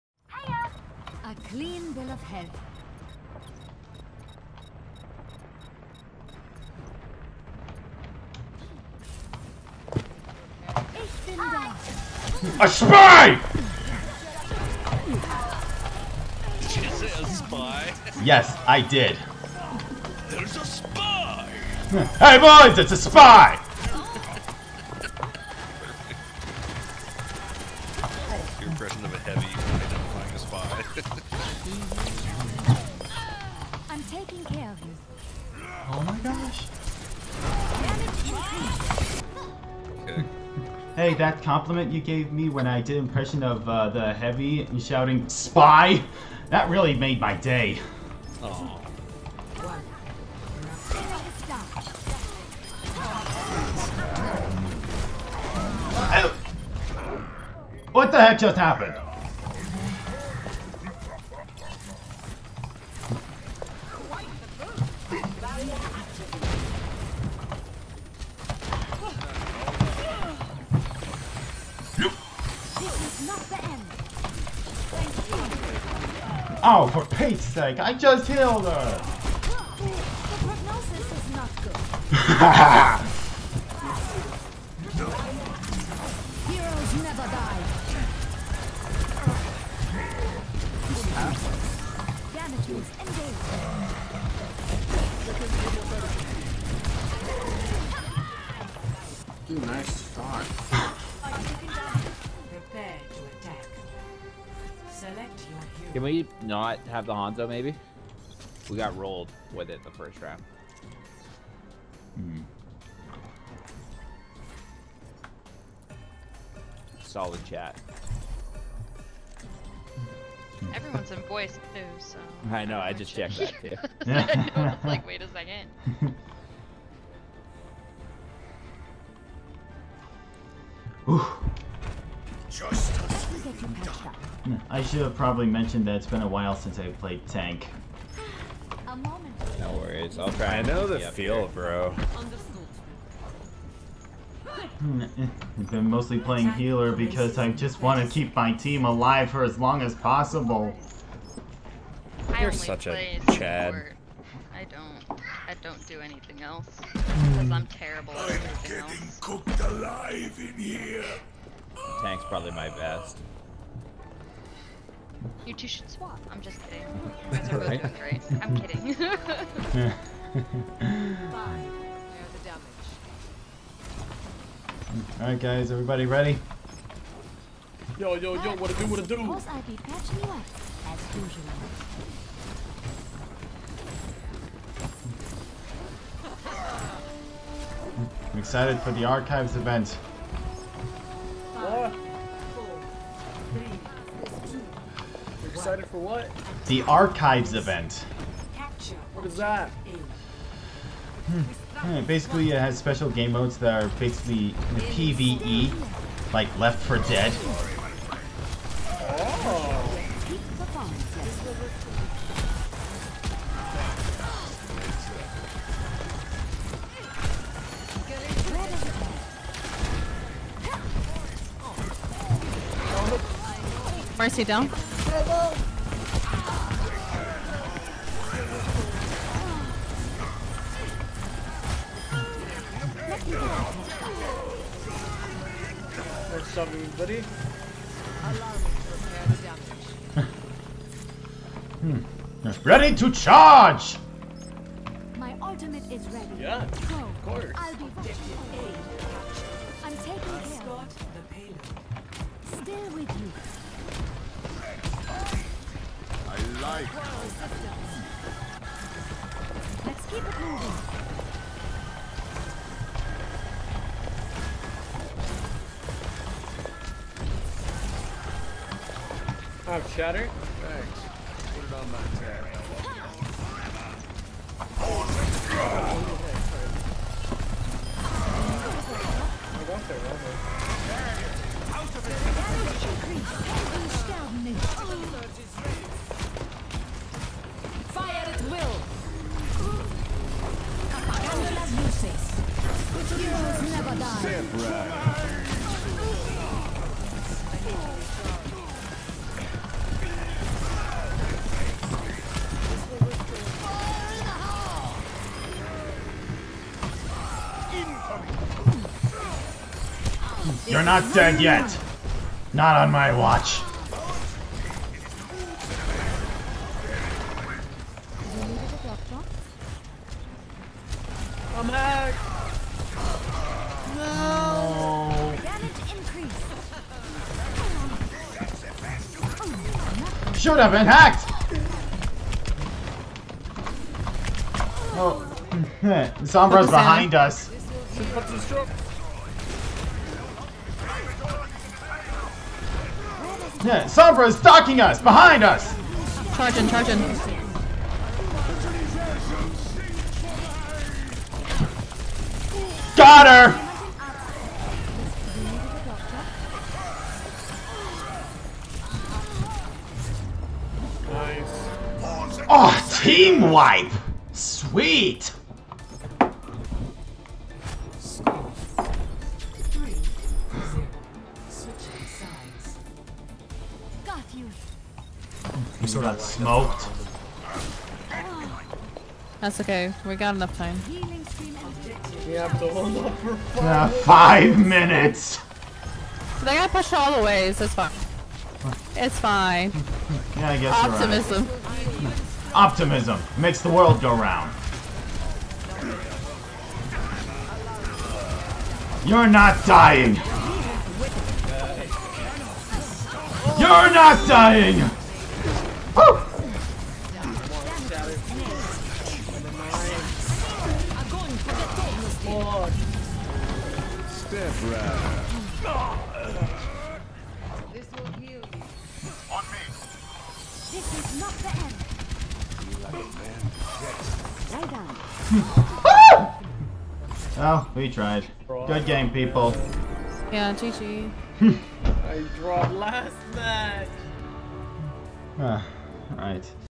Pachimari Event Stream
This video will show some of my favorite moments while doing the stream for 3 days.
Day 1: I don't remember how it went.
Day 2: It was awful. Nothing but a ton of losses.
Day 3: Things really got going and some of my favorite moments happened on this day.